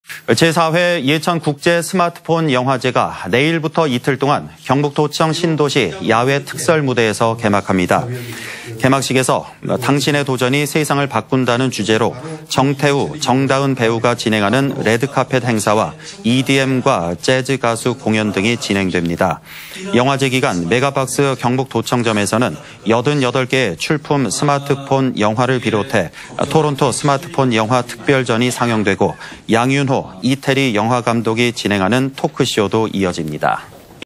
예천 국제스마트폰영화제, 오는 15일 개막 / 안동MBC
2022/10/14 09:53:22    작성자 : 김경철

제4회 예천국제스마트폰영화제가
내일(15일)부터 이틀간 경북도청 신도시
야외 특설무대에서 개막합니다.
◀ＥＮＤ▶

개막식에서 '당신의 도전이 세상을 바꾼다'는 주제로 정태우, 정다은 배우가 진행하는
레드카펫 행사, EDM과 재즈가수 공연 등이
진행됩니다.

영화제 기간 메가박스 경북도청점에서는
88개의 출품 스마트폰 영화를 비롯해
'토론토스마트폰영화 특별전'이 상영되고,
양윤호, 이태리 영화감독이 진행하는 토크쇼도 이어집니다.